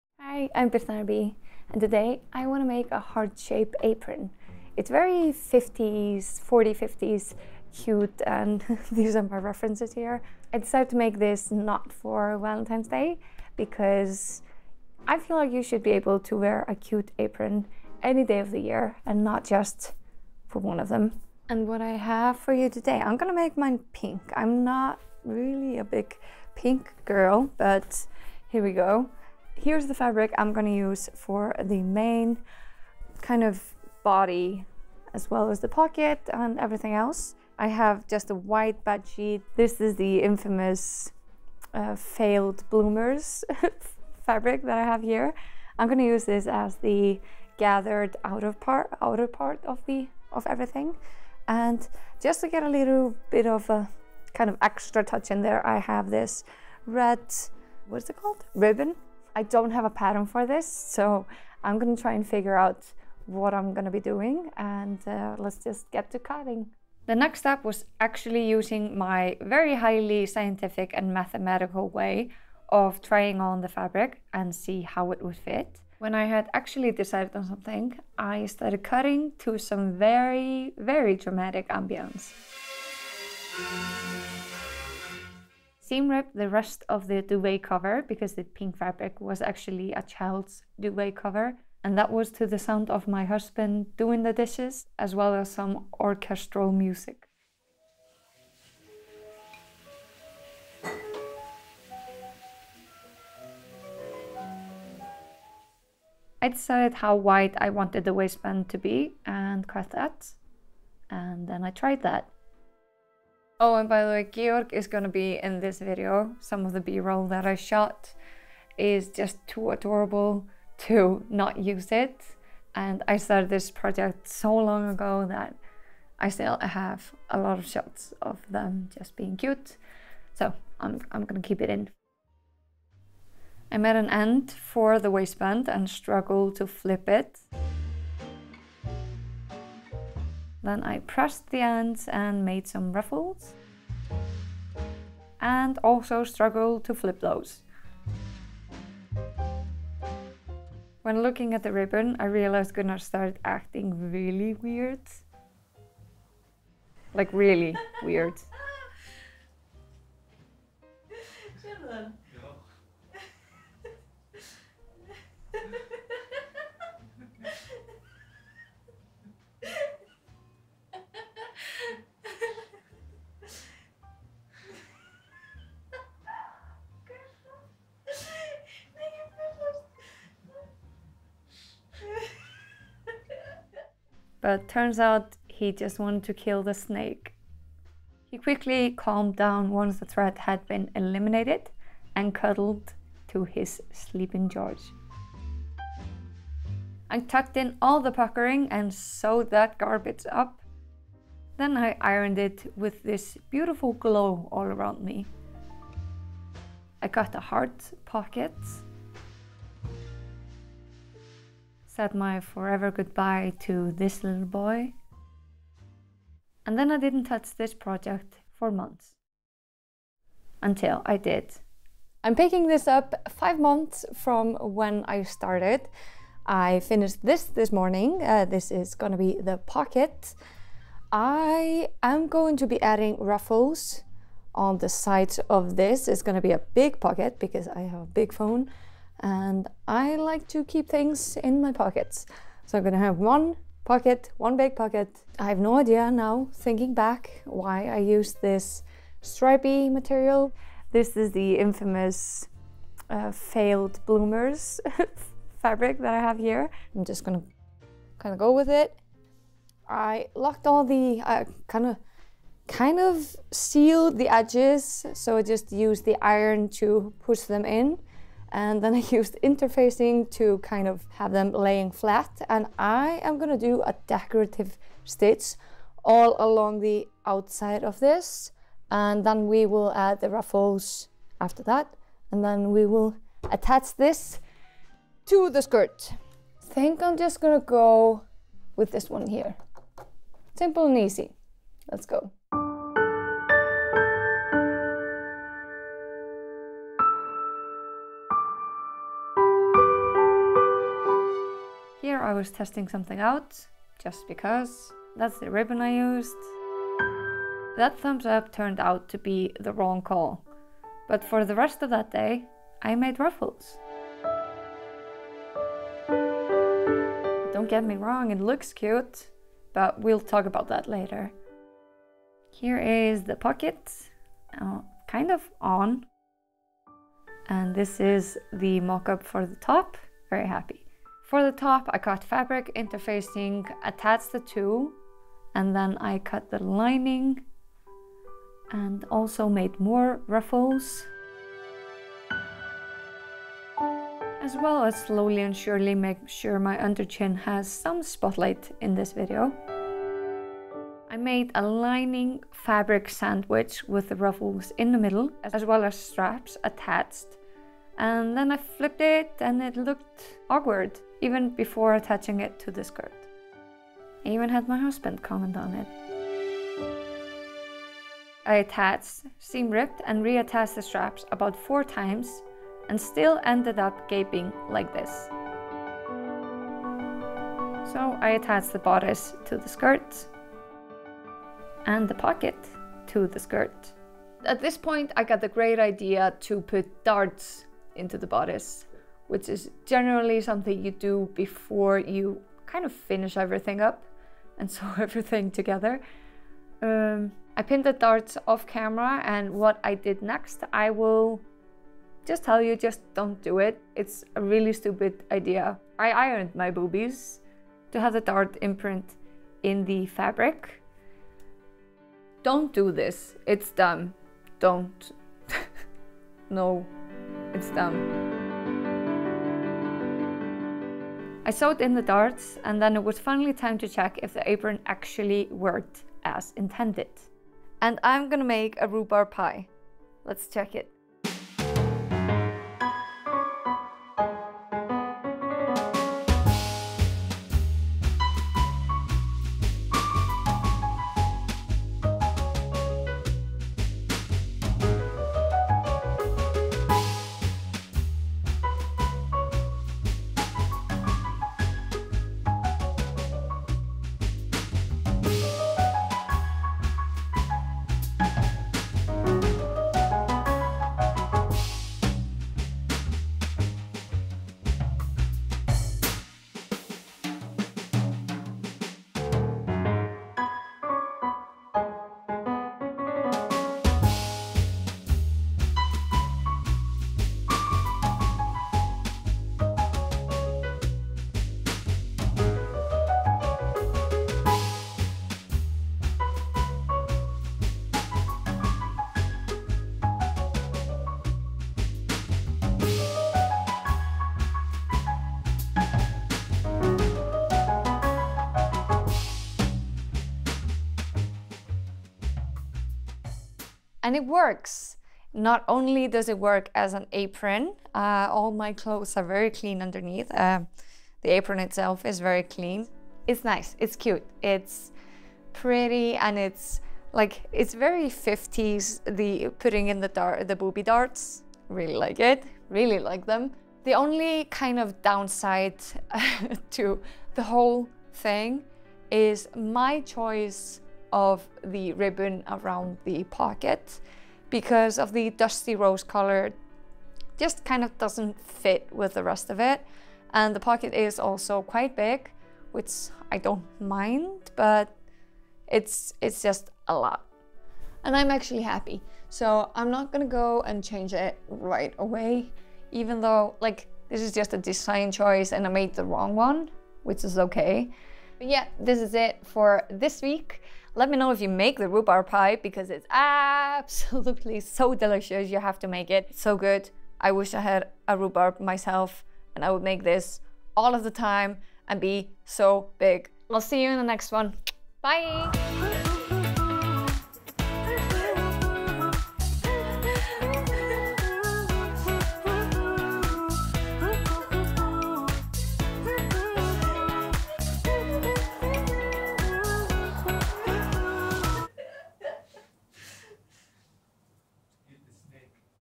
I sewed my own HEART shaped apron from a bed sheet!
In this video, I sew my own HEART shaped apron from an old children bed sheet! This apron is the perfect way to show your love for cooking and baking!
Not only is this apron cute and stylish, it's also a great way to show your love for sewing!
Now off you go and make that rhubarb pie!